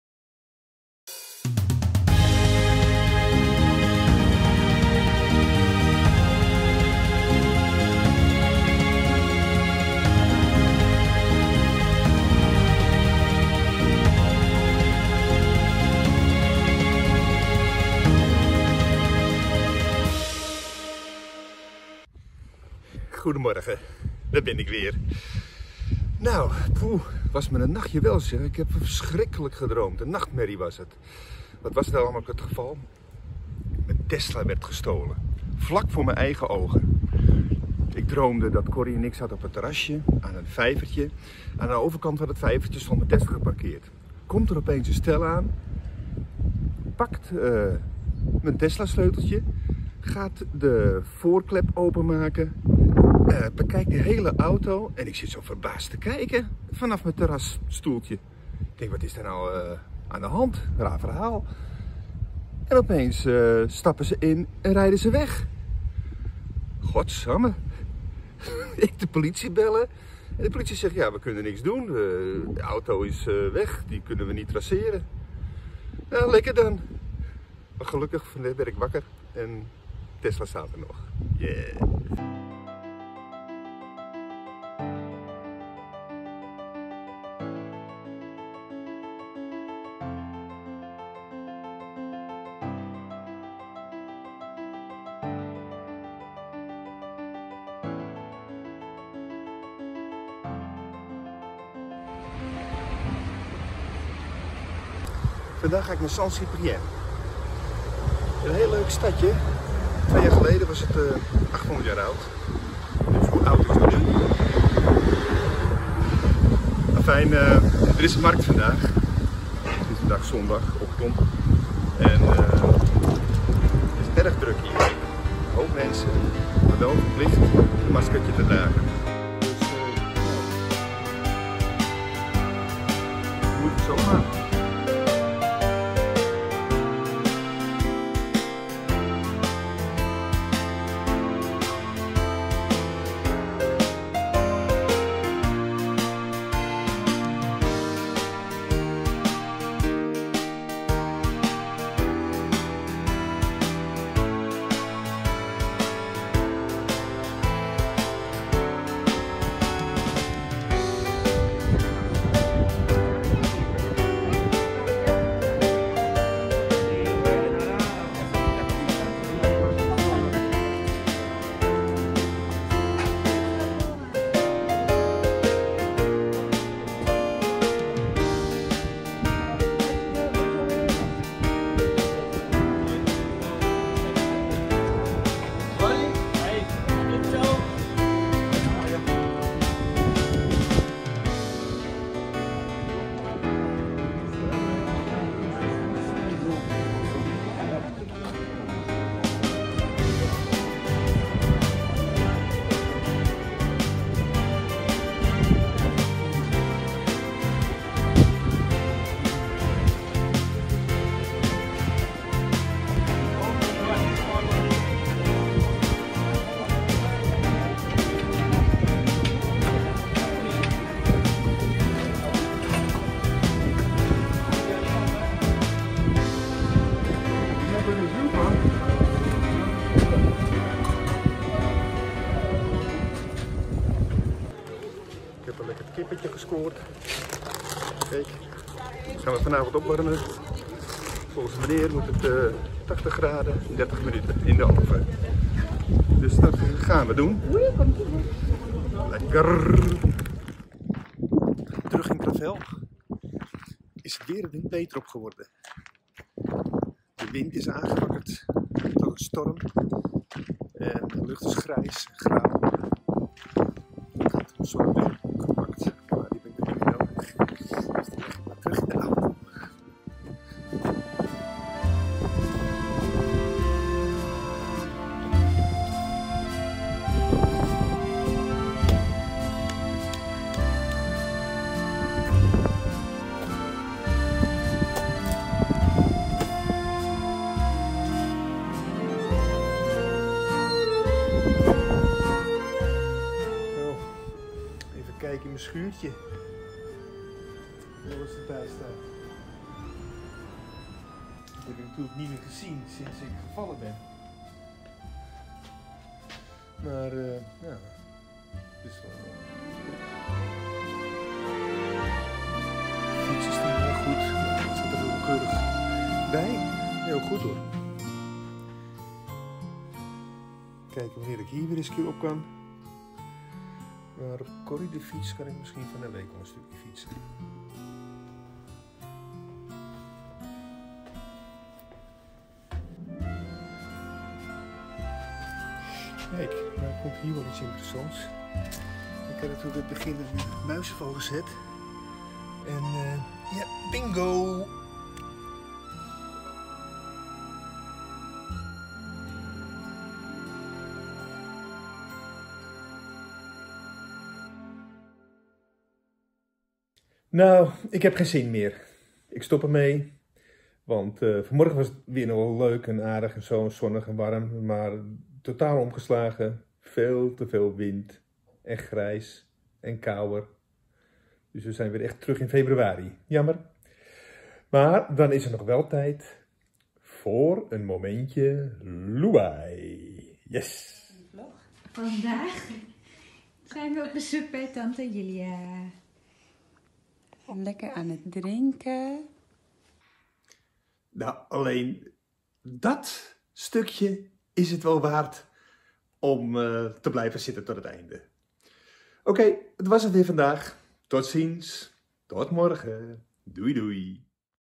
Dooie muizen, gegrilde kip en tripje naar de markt in Saint-Cyprien.
Ik loop wat rond in de tuin en vind dooie muizen in de schuur. Op de markt in Saint-Cyprien kocht ik een heerlijk gegrilde kip.
Dit keer heb ik wat zelfgecomponeerde muziek toegevoegd :)